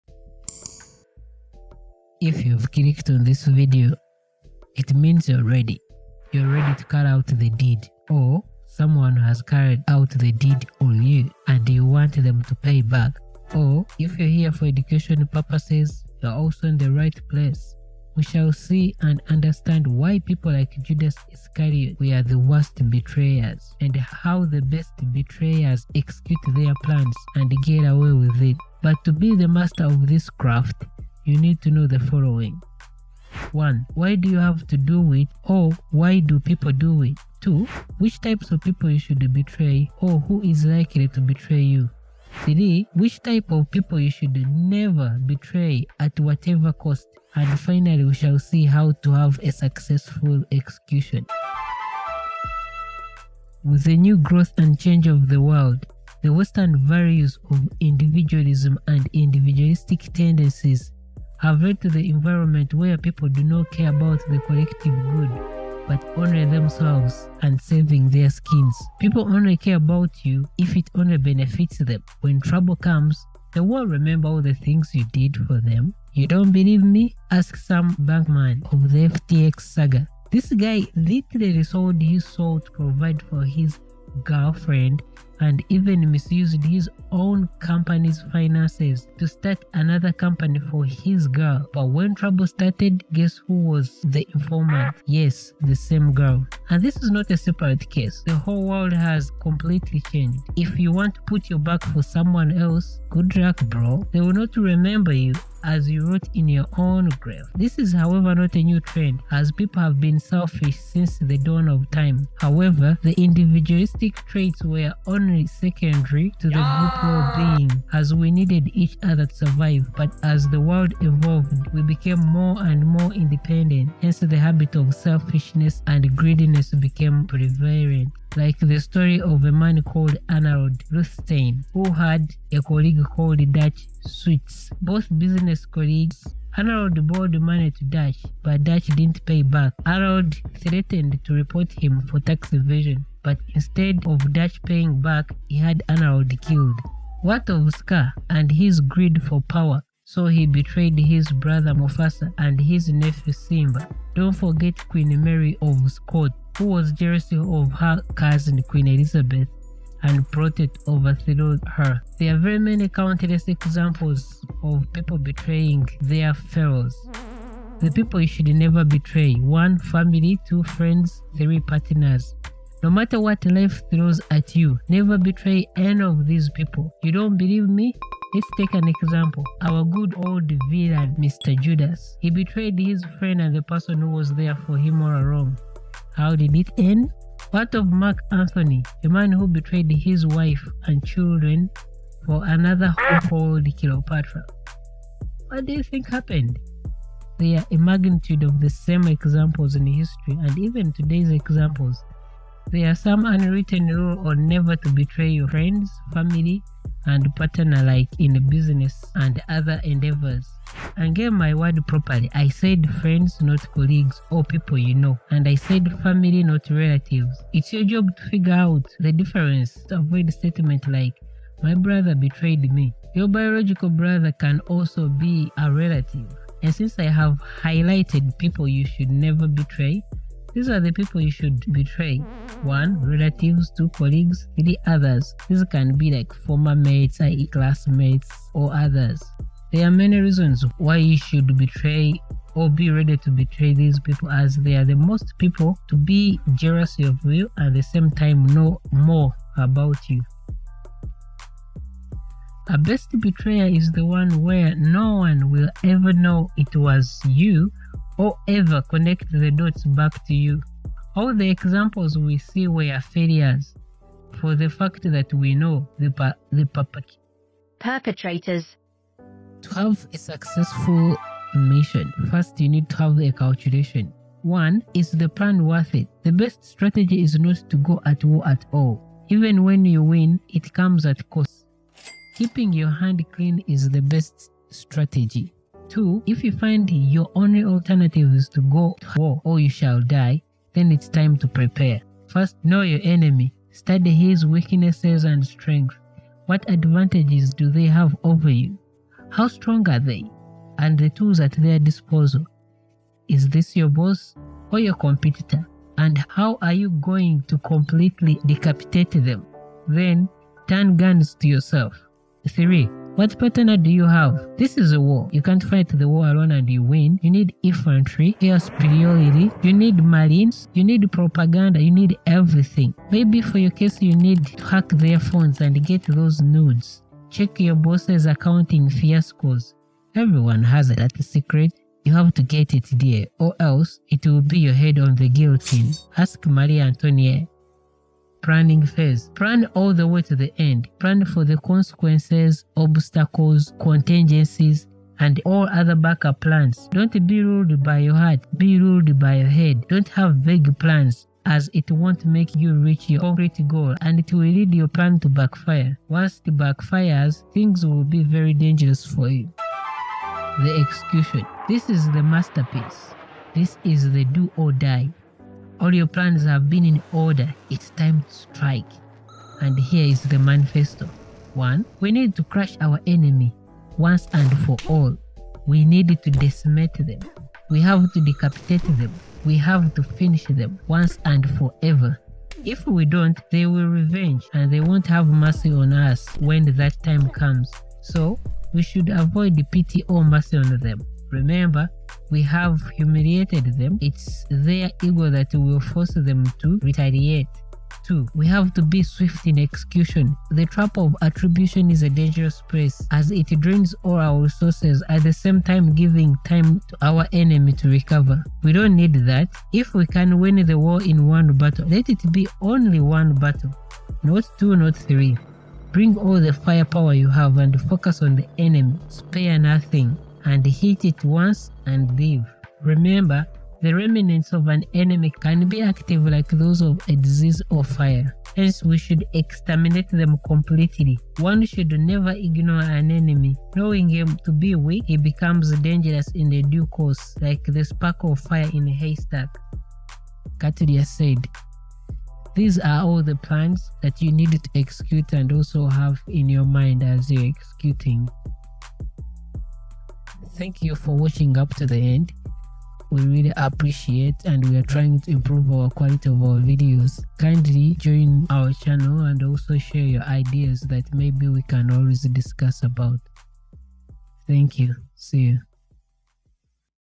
How to betray someone and get away with it
Have you ever been betrayed. Could you have avoided it. Or you want revenge?
00:00 - Intro.
00:37 - Summary
00:54 - Reasons why people betray others
01:50 -People have always been selfish
02:10 - Examples of betrayers
02:48 - The people you should never betray.
03:50 -The people you can betray
04:15 -The process
04:30 - The calculation
05:39 - Planning
06:00 -Execution.

Betrayal is one of the most painful experiences we can go through. But why do people betray us? And how can we prevent it from happening again? In this video, I explore the psychology of betrayal, the signs of a potential betrayer, and the steps to heal from the wounds of betrayal. Watch this video if you want to learn how to deal with betrayal and protect yourself from future betrayals.